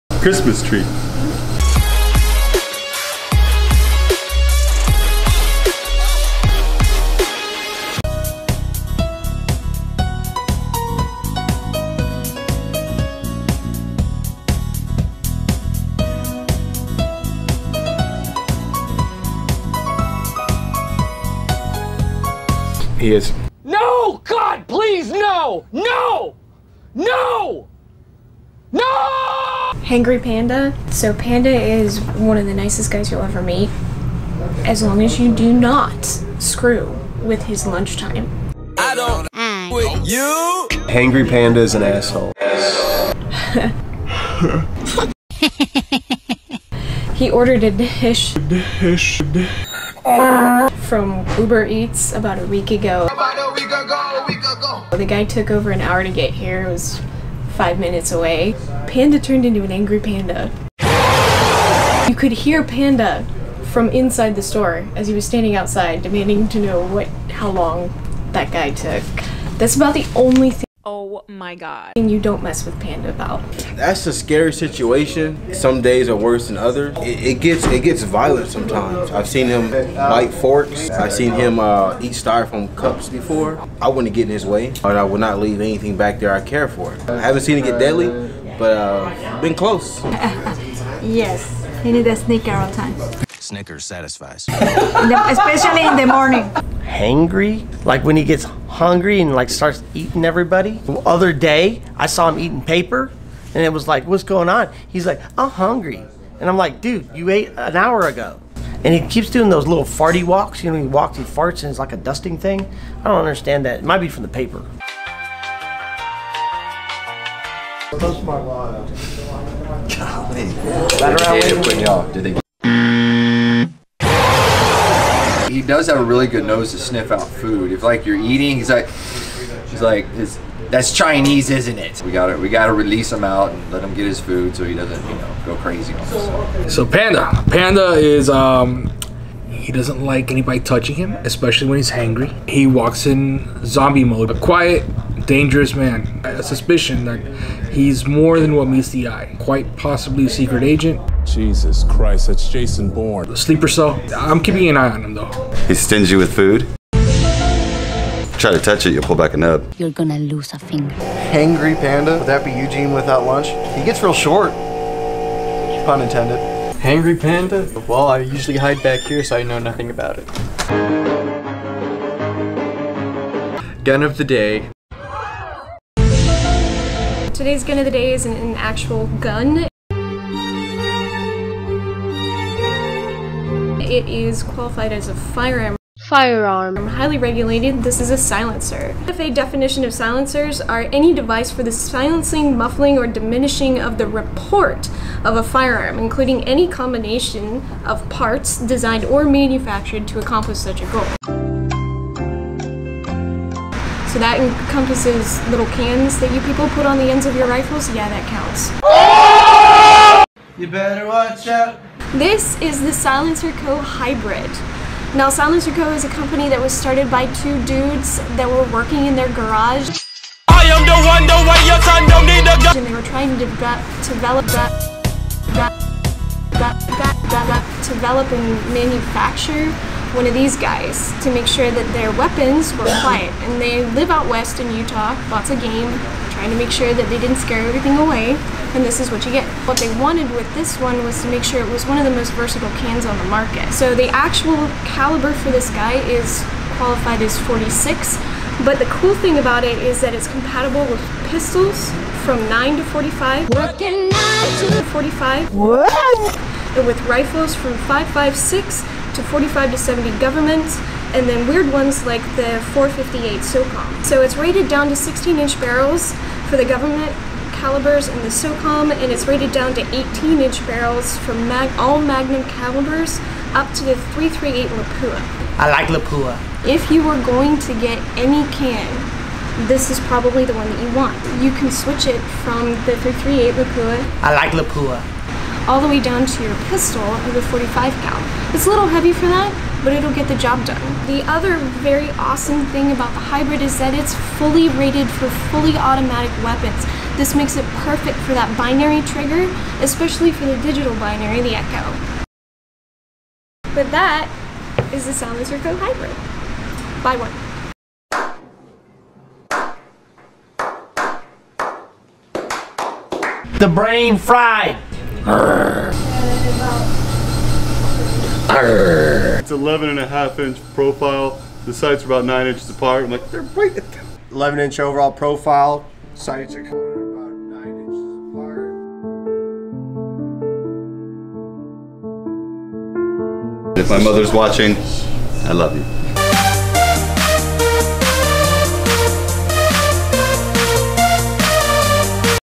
Ep.4: WE FIGHT OFF A HANGRY PANDA
Today we encounter a very hungry, angry Panda and present a not so gun-like Gun of the Day